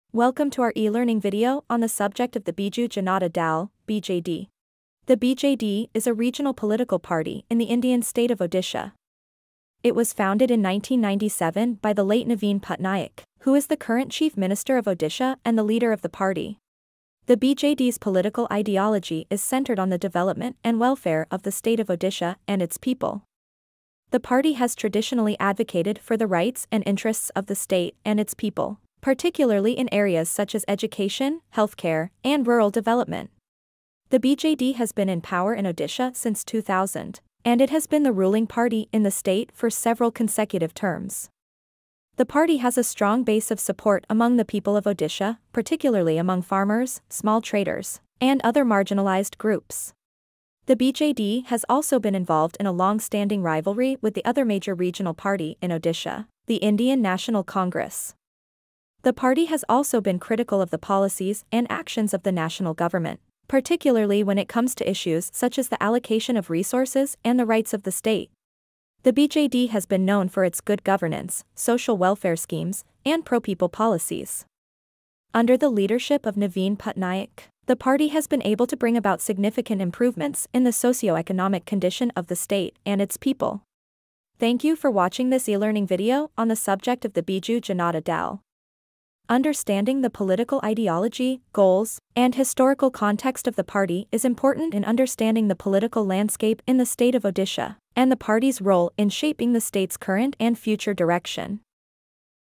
Biju Janata Dal: Understanding the Political Party and its Impact on Odisha, India
Learn about the Biju Janata Dal (BJD), a regional political party in Odisha, India. This e-learning video will cover the history, principles, and impact of the BJD on the state's politics. Discover the key figures and events that shaped the party and its ideology, as well as its relationship with other political movements and governments. By the end of this video, you will have a deeper understanding of the Biju Janata Dal and its role in shaping the political landscape of Odisha.